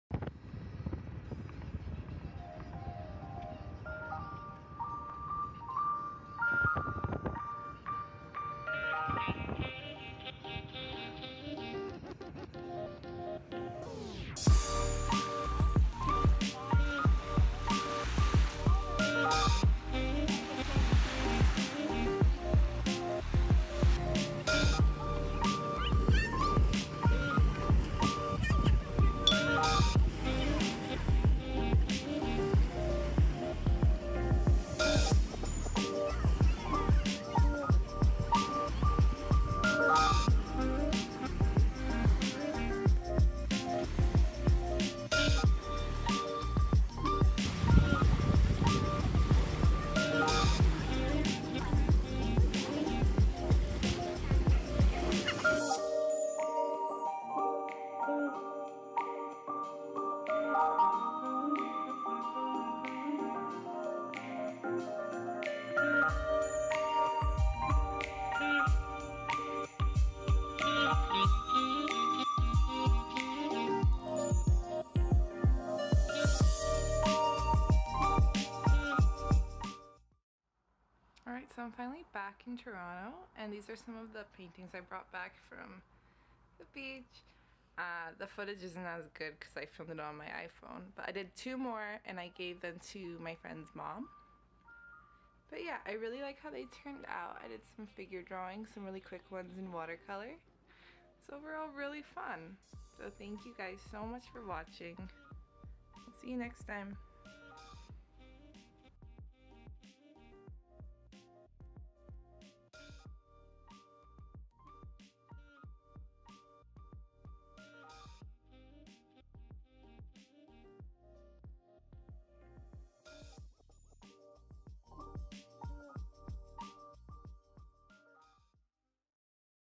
Plein Air Adventures // at the cottage
Plein Air painting on the beach while visiting my friends cottage!

Socials